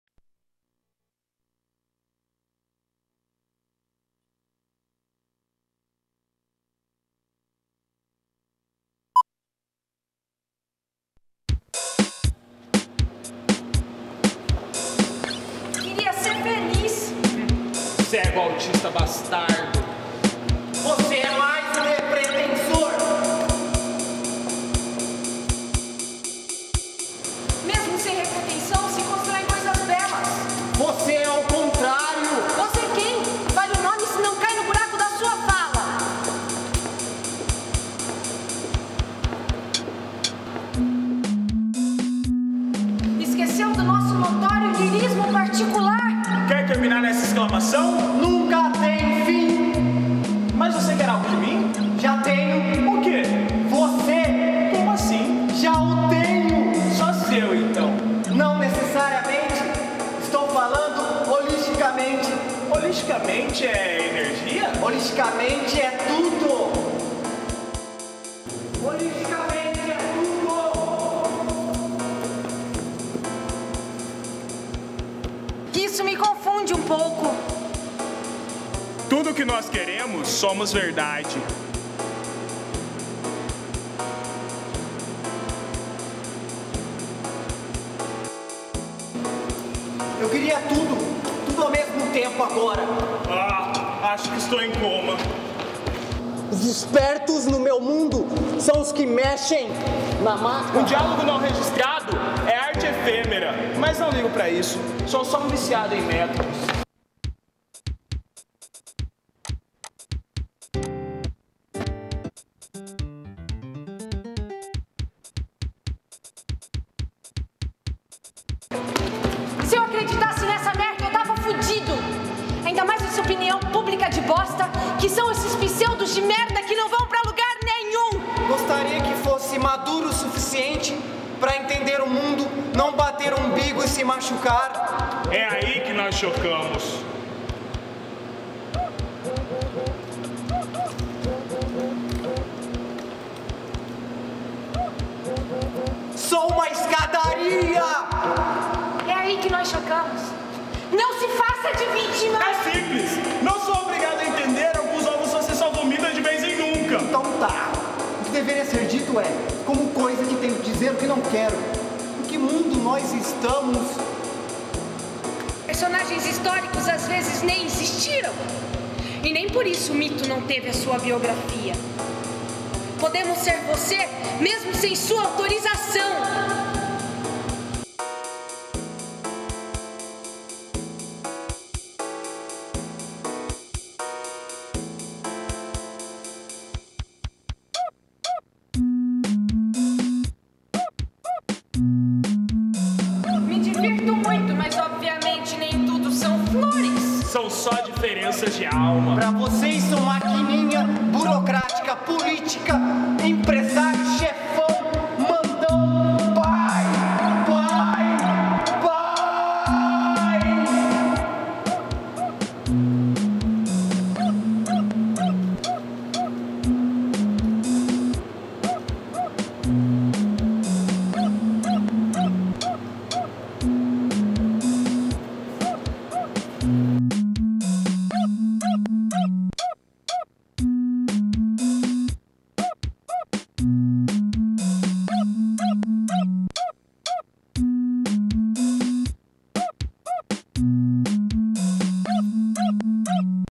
Cidade Desencantada - Copiado e Colado
Antes de mais nada, Cidade Desencantada  Copiado e Colado (2005) é um vídeo de experimentação visual-auditiva. ABC, ou seja, Arthur, Bruna e Caio, o criaram a partir de fragmentos de diálogos extraídos do banco de idéias Mutivu Vidal (um coquetel molotov de referências, inclusive e especialmente do ócio criativo no MSN:arte perene, aqui sou eu subvertendo o próprio texto do vídeo). Partindo dessa prática despretensiosa, criaram um complexo e divertido (filosófico não, pelamordedio!) diálogo entre os  seus?  eus-estrangeiros e o adornaram com grafismos e trilha sonora de video-game. Entorno de quatro minutos, o trio circula por um estacionamento disparando sua deliciosa acidez.